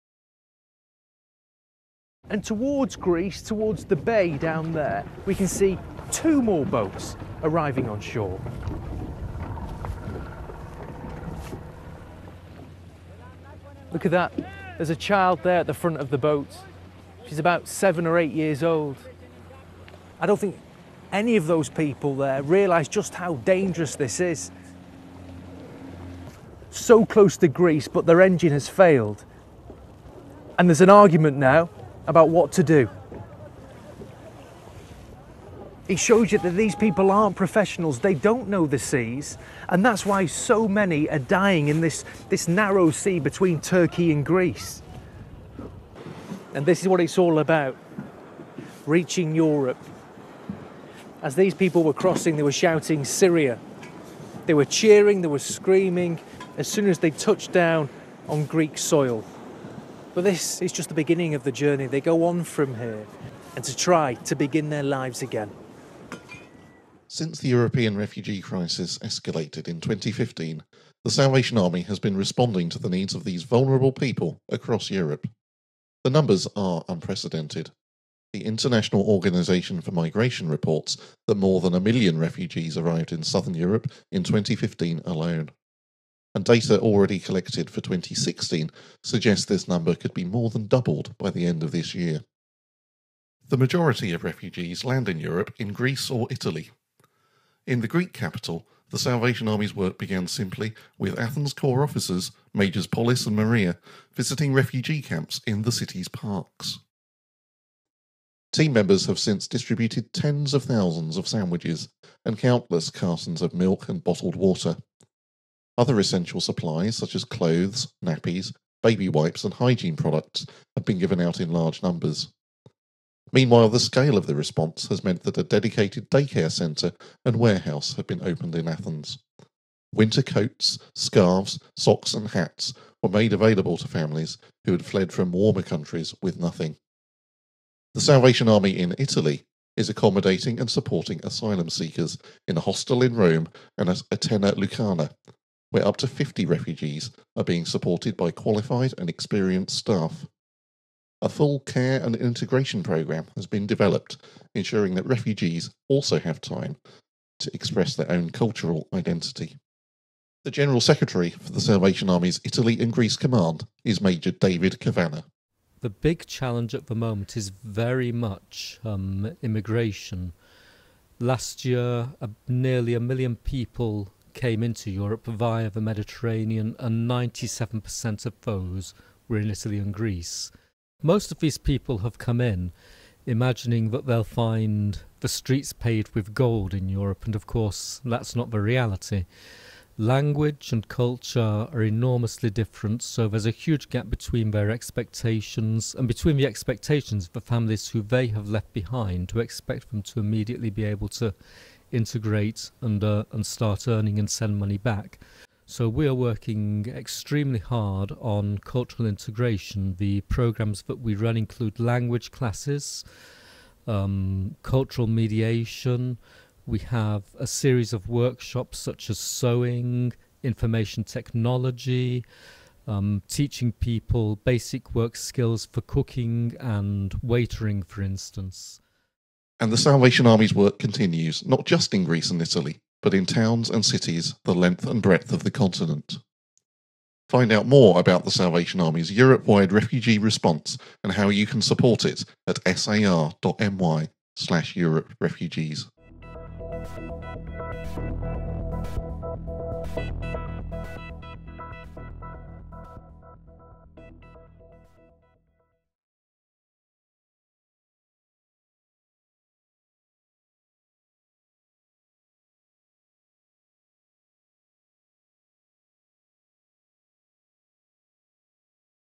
Europe Refugee Crisis update: Italy & Greece
The Salvation Army has been responding to the Europe-wide refugee crisis since it escalated in 2015. Major David Cavanagh, the church and charity's General Secretary in Italy & Greece outlines some of the challenges affecting refugees in the region and The Salvation Army's response. Learn more, and find out how to support this important work,